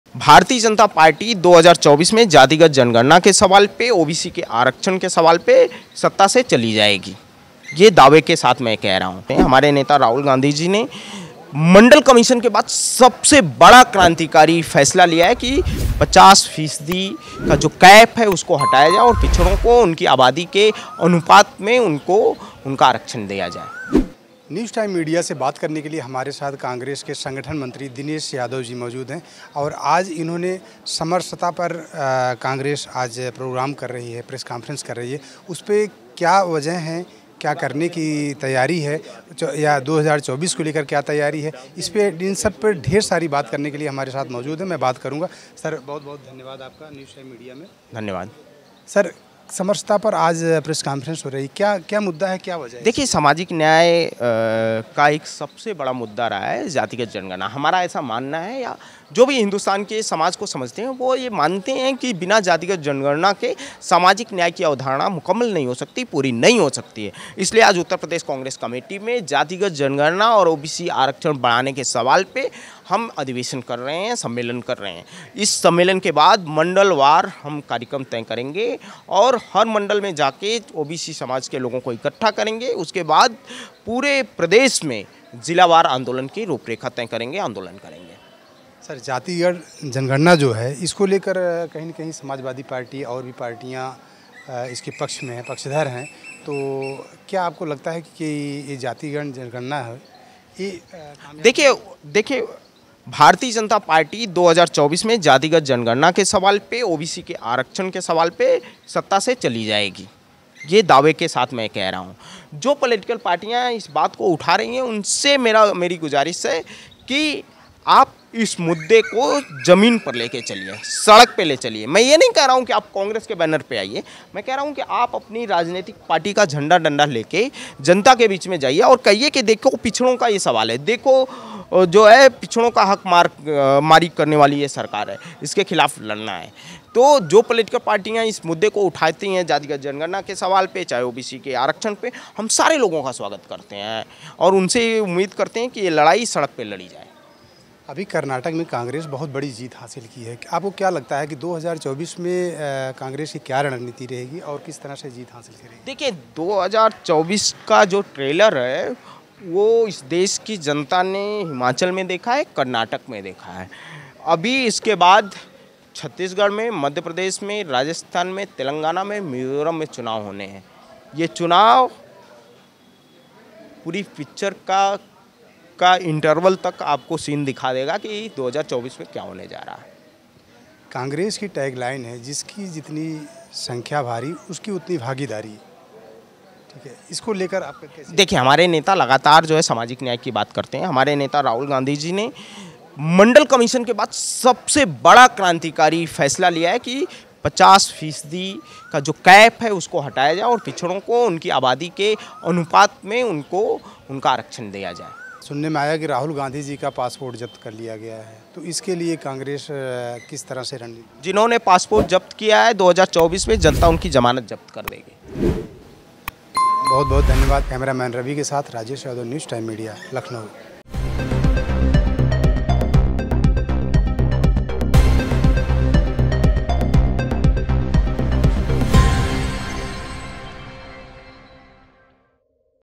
Congress ने किया पलटवार पासपोर्ट जब्त करनें वालों की 2024 में होगी ज़मानत जब्त || News Time Media
News time media पर आपको मिलेंगे राजनीति , मनोरंजन , सामाजिक जगत के मजेदार वीडियो ।